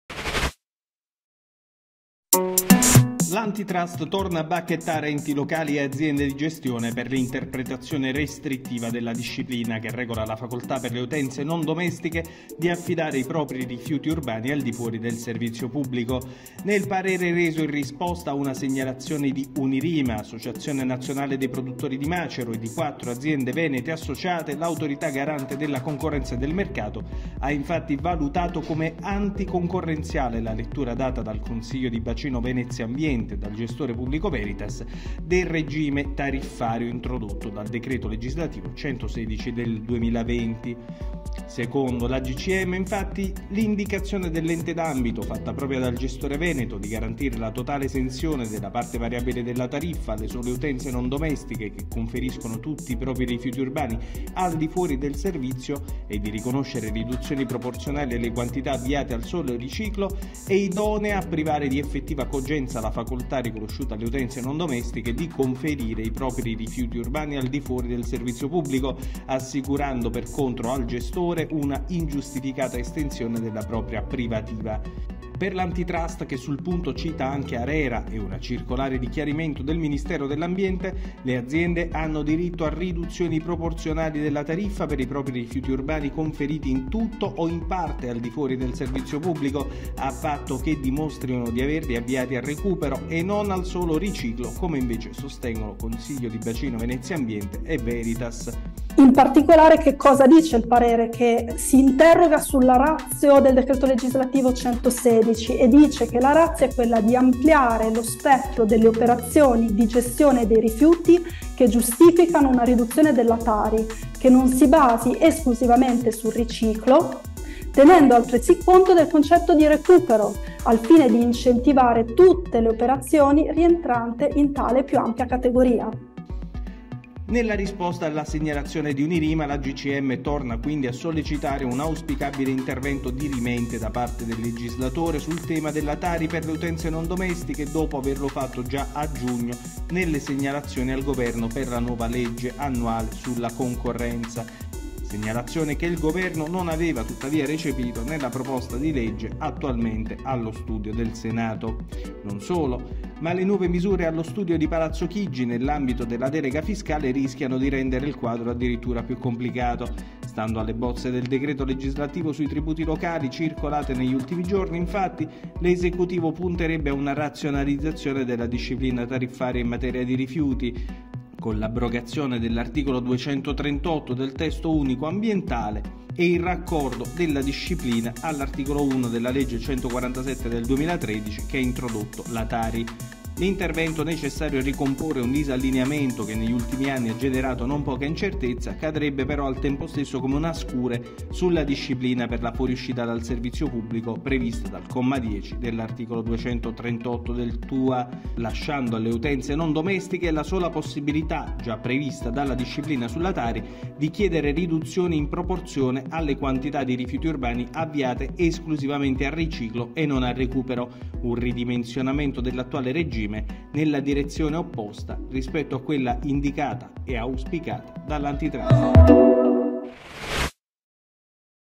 Rifiuti urbani, l’antitrust: “No a ingiustificate estensioni della privativa”
L’AGCM torna a stigmatizzare l’interpretazione restrittiva della disciplina che regola la fuoriuscita delle utenze non domestiche dal servizio pubblico di gestione dei rifiuti urbani, chiedendo un intervento “dirimente” da parte del legislatore